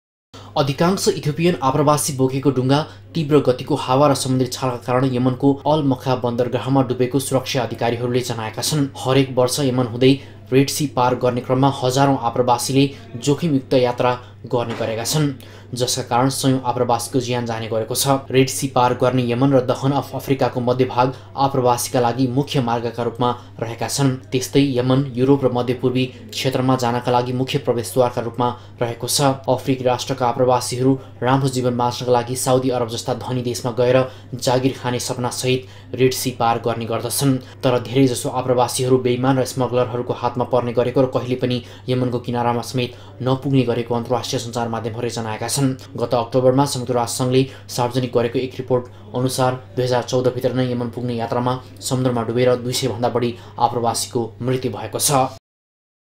उत्तरी यमनको समुन्द्र तटमा दुर्घटनामा कम्तिमा ७० जनाको मृत्यू - NEWS24 TV
यमन । यमनको उत्तरी समुन्द्री तटमा आप्रबासी बोकेको डुगां दुर्घटना हुदा ७० जनाको मृत्यु भएको छ । डुंगामा अफ्रिकन आप्रबासी रहेको यमनका अधिकारीहरुले जनाएका छन ।
अधिकांश इथोपियन आप्रबासी बोकेको डुंगा तीब्र गतिको हावा र समुन्द्री छालका कारण यमनको अल मखा बन्दरगाहमा डुबेको सुरक्षा अधिकारहरुले जनाएका छन । हरेक बर्ष यमन हुदै रेड सि पार गर्ने क्रममा हजारौ आप्रबासीले जोखिम युक्त यात्रा गर्ने गरेका छन । जसका कारण सयौं आप्रबासीको ज्यान जाने गरेको छ । रेड सि पार गर्ने यमन र द हर्न अफ अफ्रिकाको मध्य भाग आप्रबासीका लागि मुख्य मार्गका रुपमा रहेका छन । त्यस्तै यमन, युरोप र मध्य पुर्वी क्षेत्रमा जानका लागि मुख्य प्रवेशद्धारका रुपमा रहेको छ । अफ्रिकी राष्ट्रका आप्रबासीहरु राम्रो जीवन बाच्नका लागि साउदी अरब जस्ता धनी देशमा गएर जागिर खाने सपना सहित रेड सि पार गर्ने गर्दछन । तर धेरै जसो आप्रबासीहरु बेइमान र स्मग्लरहरुको हातमा पर्ने गरेको र कहिल्यैपनि यमनको किनारामा समेत नपुग्ने गरेको अन्तराष्ट्रिय संचारमाध्यमहरुले जनाएका छन । गत अक्टोबरमा संयुक्त राष्ट्रसंघले सार्बजनिक गरेको एक रिपोर्ट अनुसार २०१४ भित्रनै यमन पुग्ने यात्रामा समुन्द्रमा डुबेर २ सय भन्दा बढी आप्रबसीको मृत्यु भएको छ ।